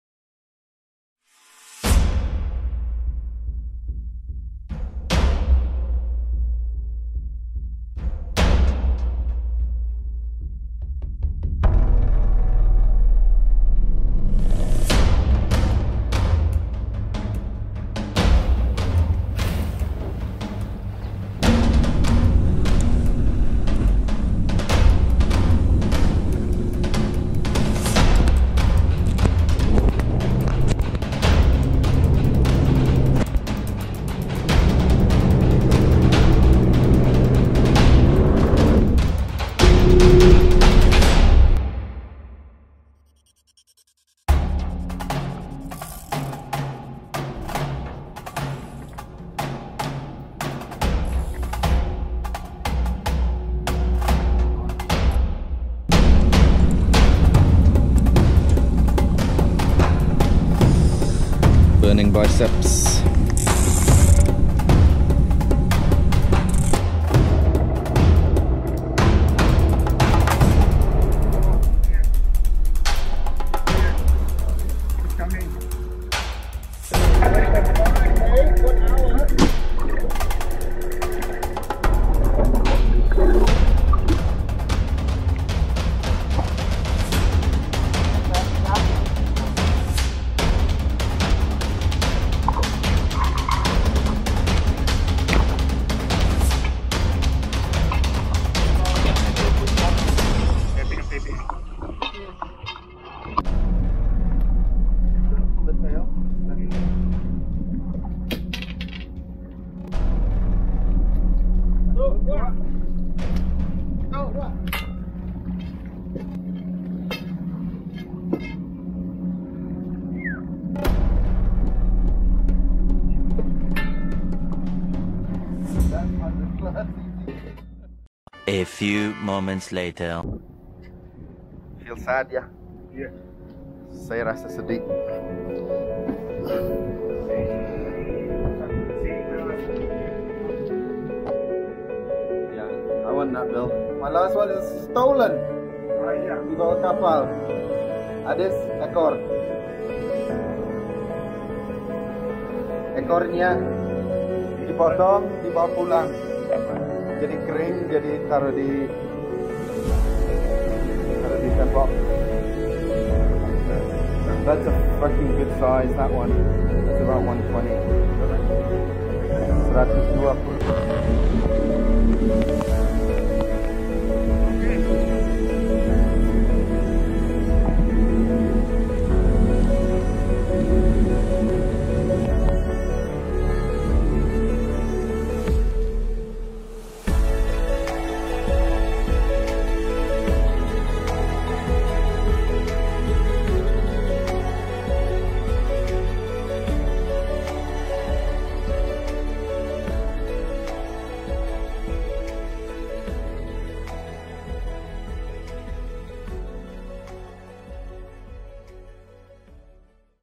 Nordhavn 8610 Koonoona Catch Blue Marlin
From Wakatobi to bali after we have a Diving trip with the owner. On the second day early in the morning nature surprised us again with 120 more or less blue marlin. December 16, 2021